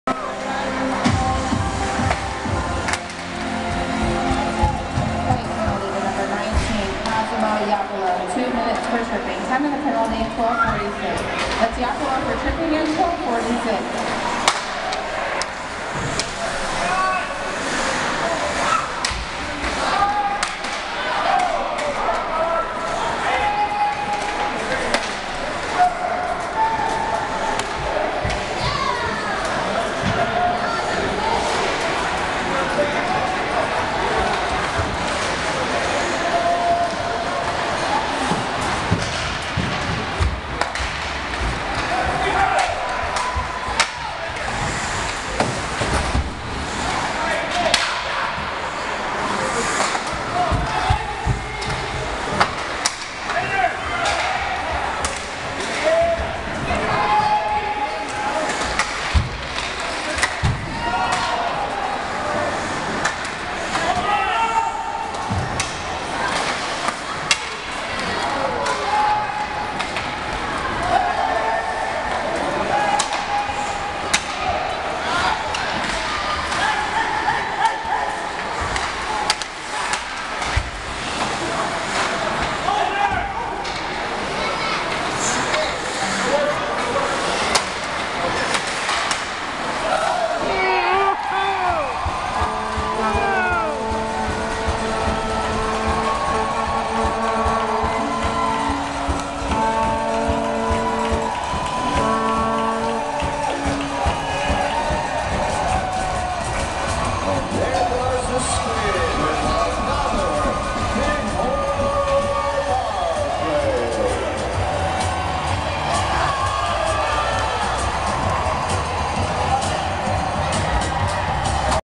Bighorns hockey
Another third period power play goal in the game on 11/29/2013 against the Gillette Wild at the Helena Ice Arena...shortly after this goal the starting goalie was replaced and the final score was 6-1 in favor of Helena